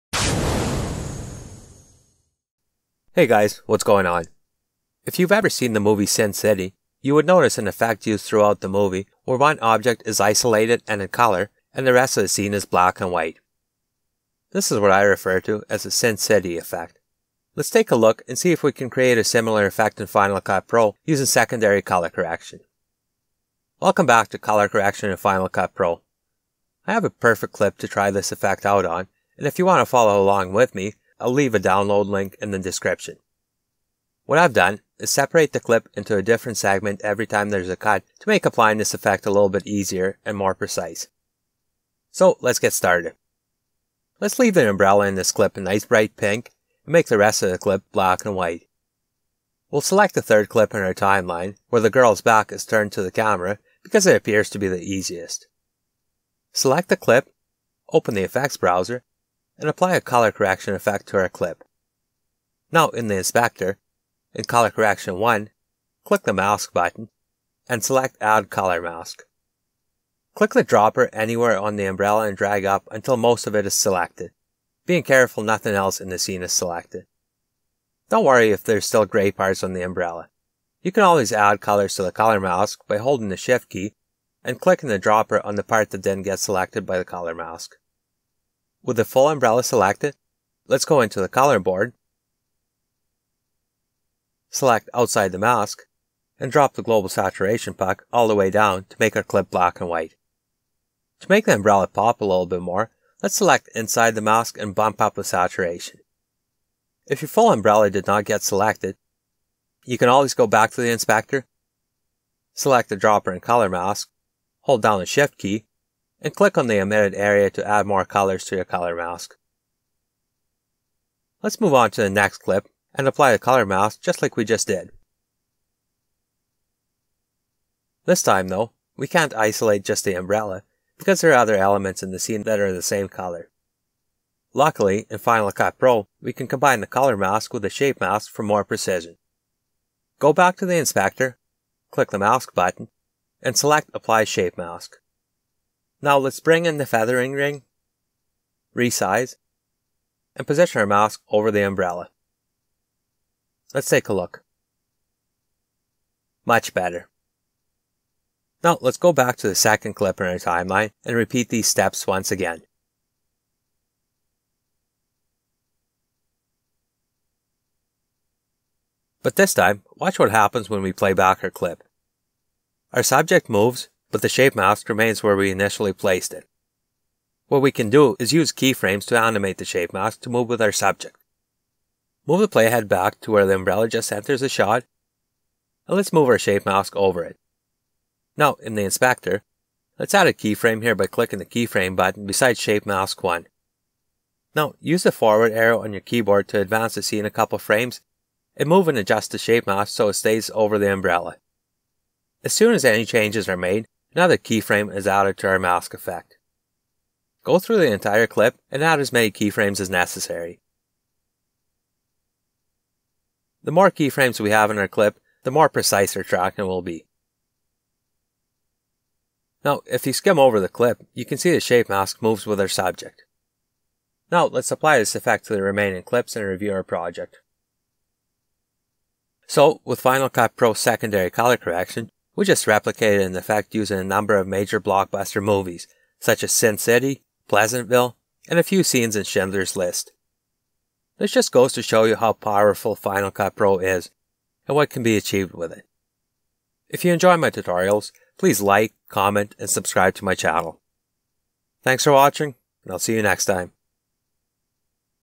Sin City Effect - FCPX 10.3 Tutorial 2017
Create Sin City or Pleasantville like effect using secondary color correction in Final Cut Pro 10.3, 2017 FCPX Tutorial. If you’ve ever seen Sin City, Pleasantville, Schindler's List etc., you would notice an effect used throughout the movie, where one object is isolated and in color, and the rest of the scene is black and white. This is what I refer to as the Sin City Effect. Lets take a look and see if we can create a similar effect in Final Cut Pro, using secondary color correction.